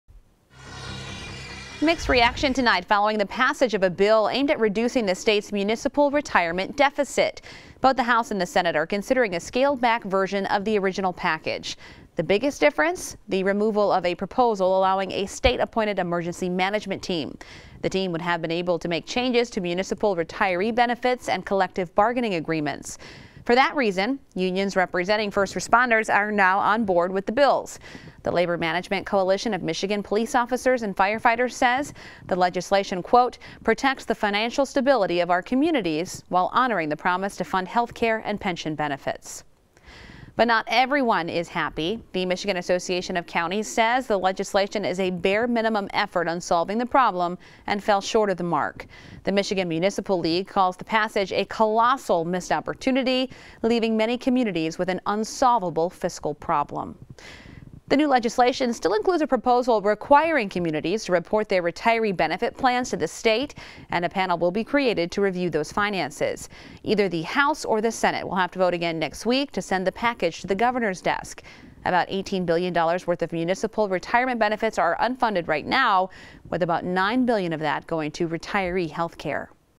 Lawmakers pass retirement bills after changes
Michigan lawmakers have approved legislation aimed at underfunded municipal retirement plans, after deciding to abandon a contentious proposal for state intervention in communities that are unwilling or unable to resolve their liabilities.